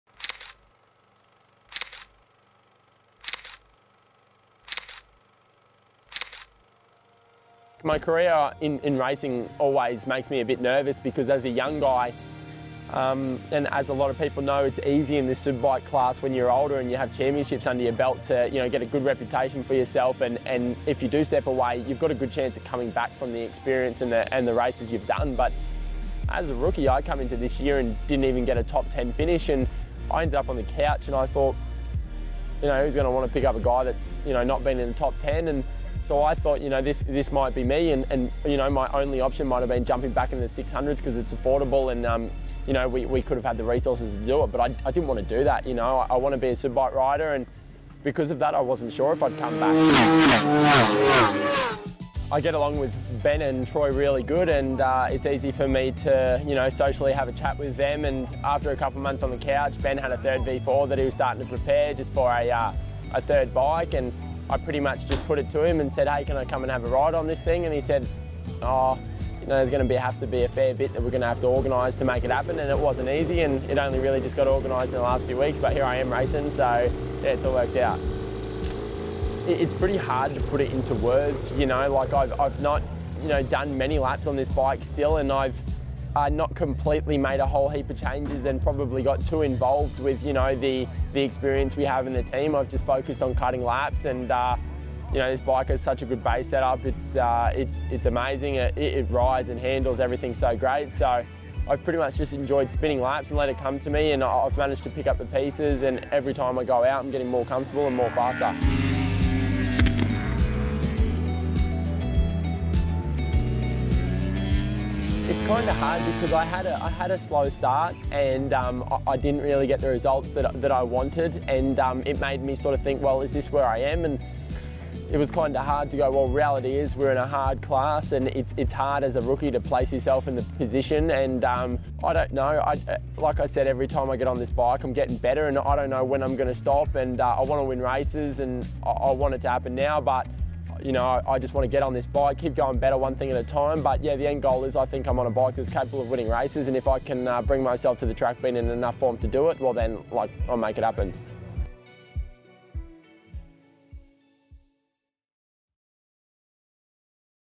BTS: Broc Pearson back on track
It was a strong comeback to the Mi-Bike Australian Superbike Championship (ASBK) for Broc Pearson at Morgan Park, with the premier class rookie scoring his strongest results of the season to date on debut with DesmoSport Ducati. In this latest BTS film, the 21-year-old discusses the lifeline provided on the V4 R and his return to racing.